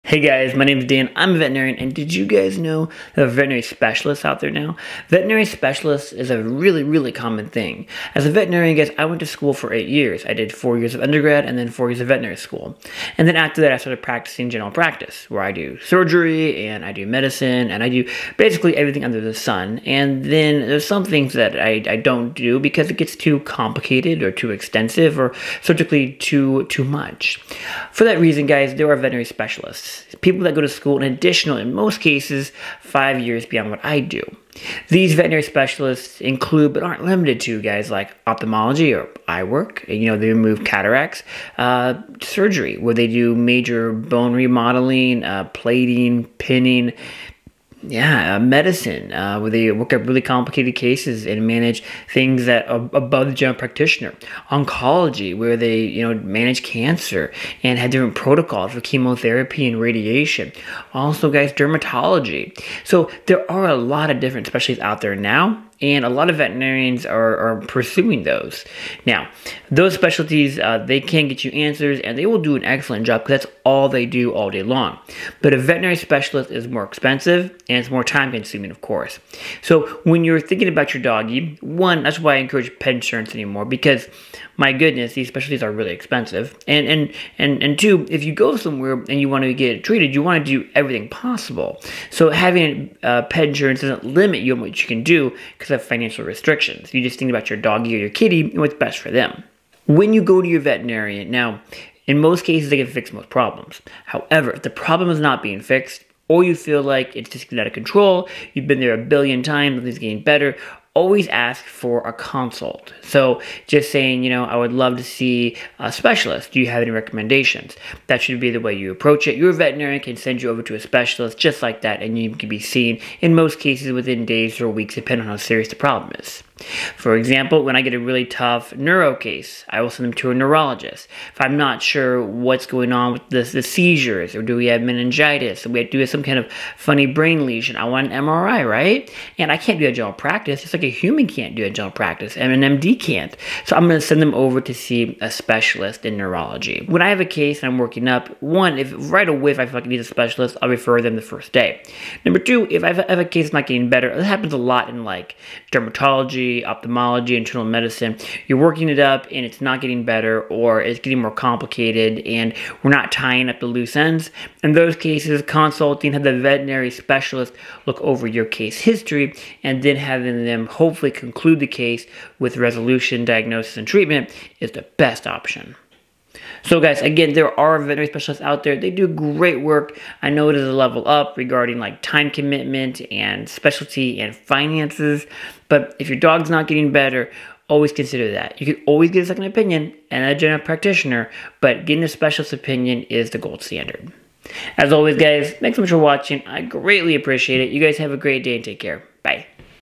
How Veterinary Specialists Work
Hi everyone,

My name is Dan, and I am a veterinarian.  Today I am talking about veterinary specialists.  As a veterinarian I went to school for 8 years after high school.  I got my bachelors and then my doctor of veterinary medicine or DVM.

After completing veterinary school, there is the option to specialize.  This option is become far more common too.  A veterinarian can pick a specialty like surgery or neurology.  Of course, general practice vets like my self do everything still, but if we need a little or lot of help on a case we will consult with a specialist.

I most cases your veterinarian will send over the medical chart to the veterinary specialist referral hospital.  The specialists that is needed will review your veterinarians medical notes and then complete a physical exam and diagnostics on your pet.  The specialists may fix the problem right then and there or work with hour vet to manage the medical concern.

If you feel like you veterinarian needs help because your pet is not getting better, always ask for a consult with a specialist.

-Dr. Dan